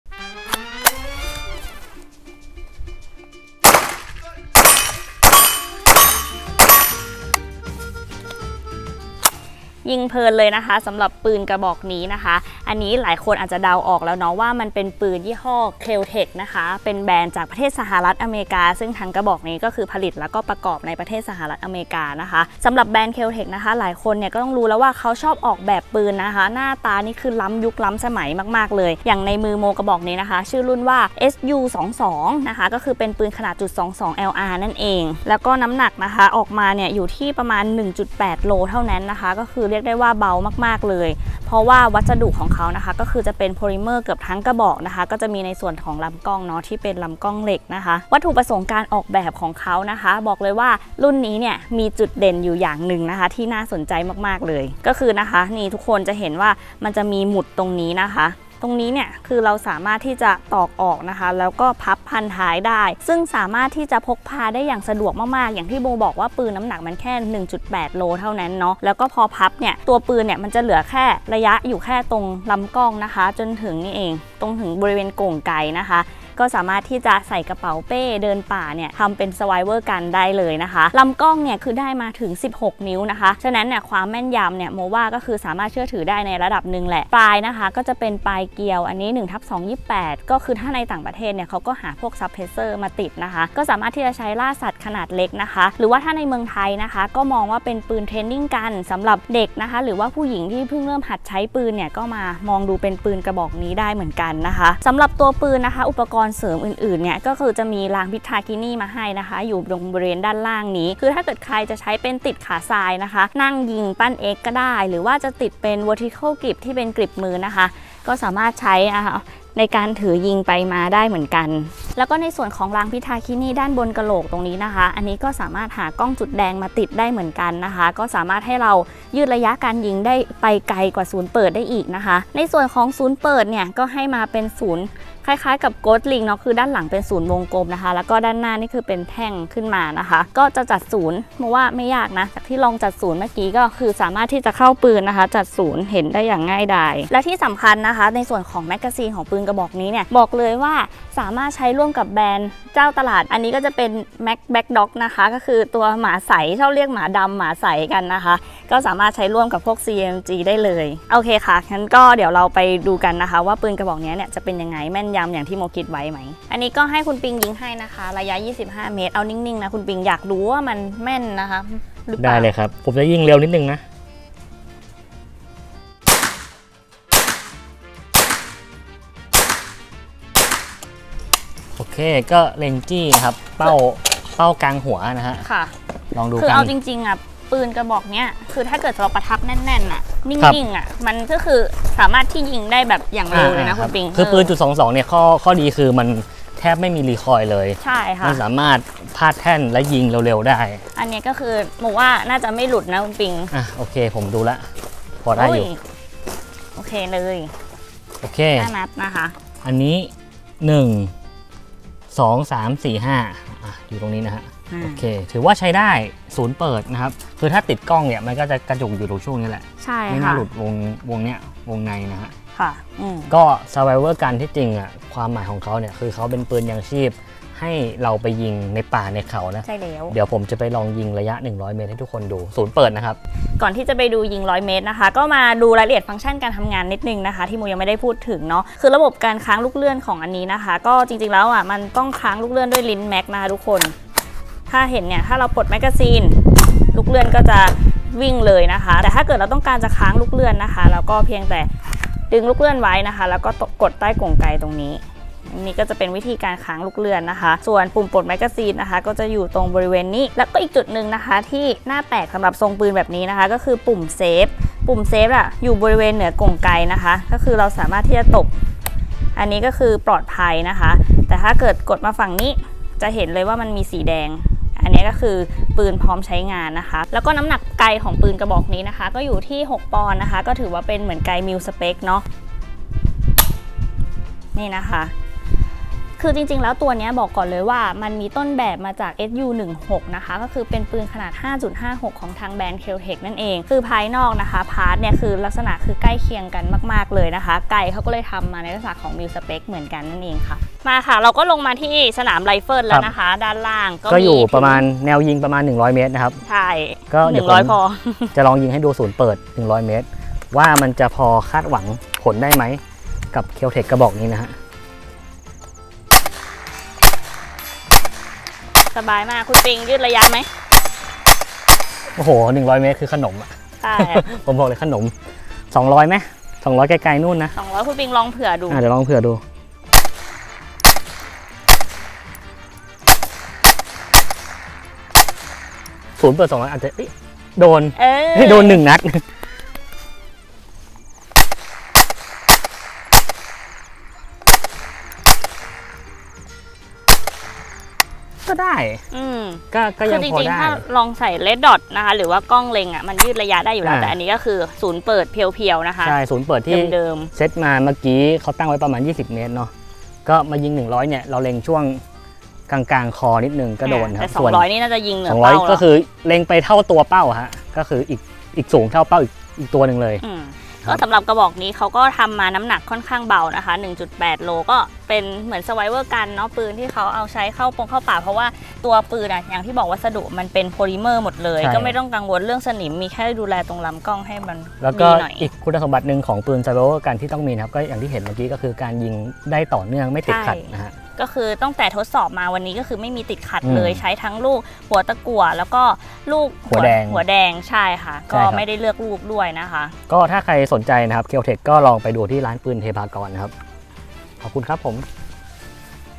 [ChannelMo] รีวิวปืน Keltec SU-22 ตอบโจทย์คนที่หาปืนลูกกรด .22lr ทรงใหม่ๆ
0:00 Intro
0:33 SU-22
2:45 25 Meters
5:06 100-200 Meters

***ปืนที่นำมาถ่ายทำทุกกระบอกเป็นปืนที่มีทะเบียน และ ใบอนุญาตครอบครองถูกต้องตามกฎหมาย**
***ไม่ขายกระสุนและอาวุธปืนทุกชนิด***